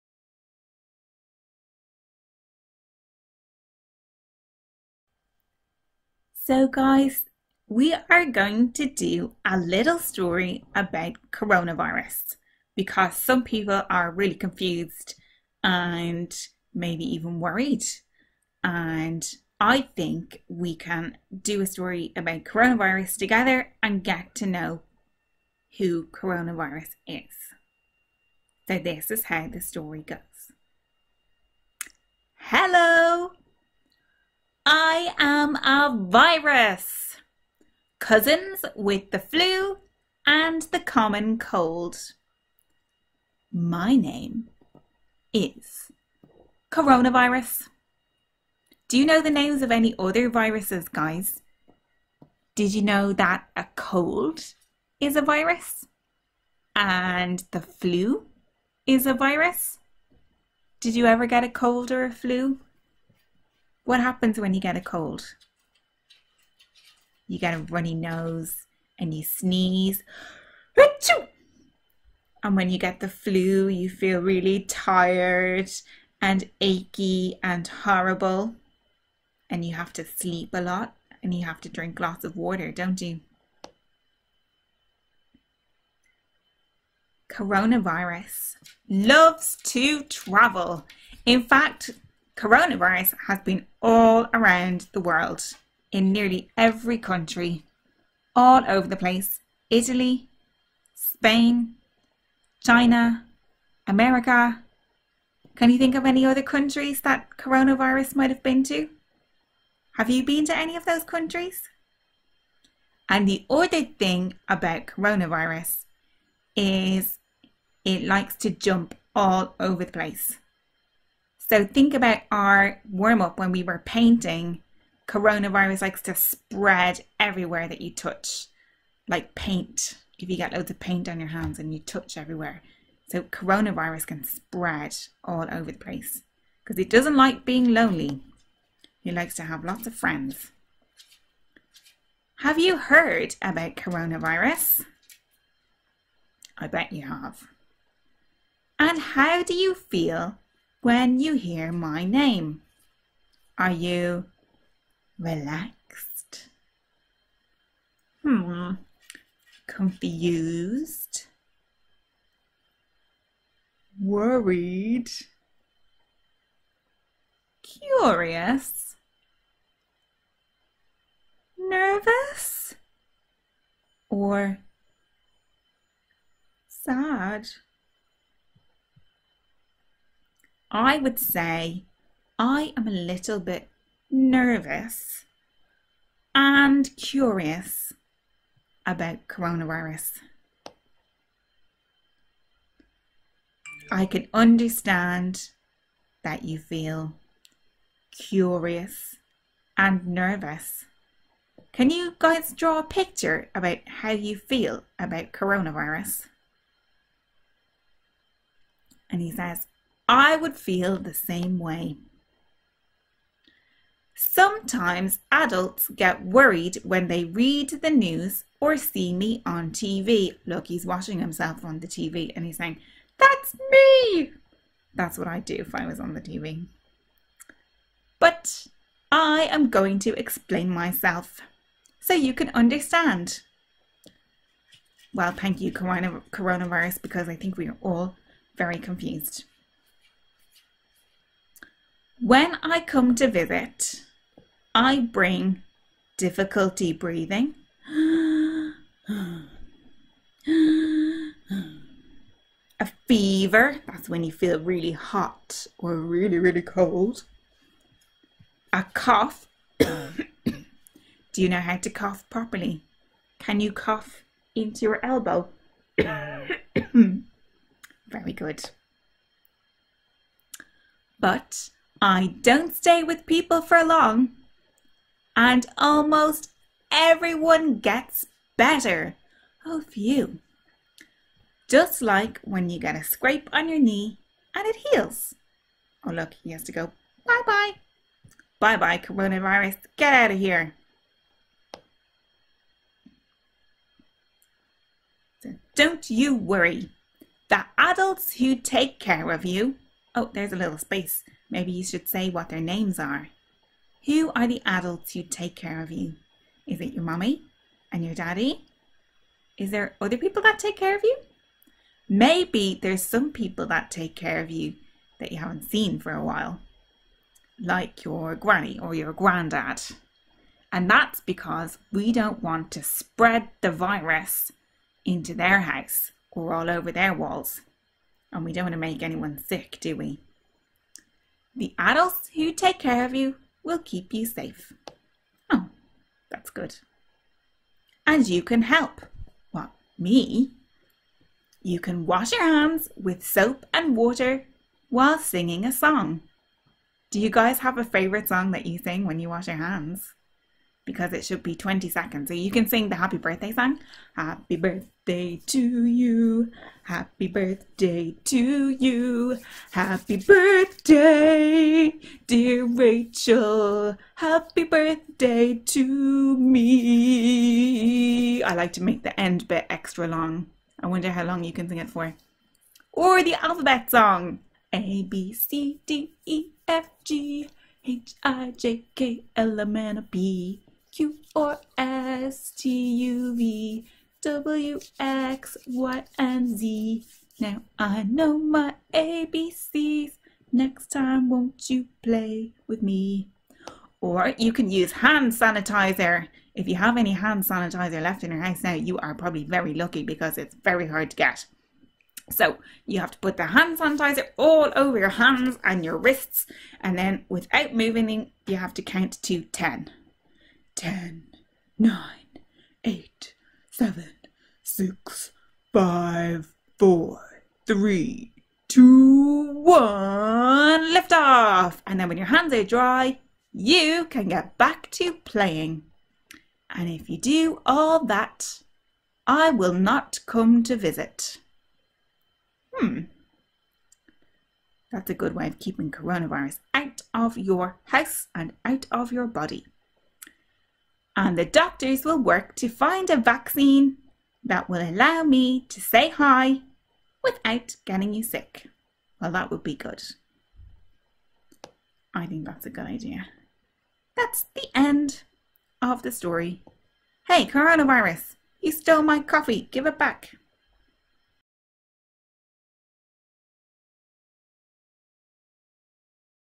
Coronavirus for Children
The Coronavirus story by Manuela Molina read by Rachel Lally.
Help children understand the coronavirus.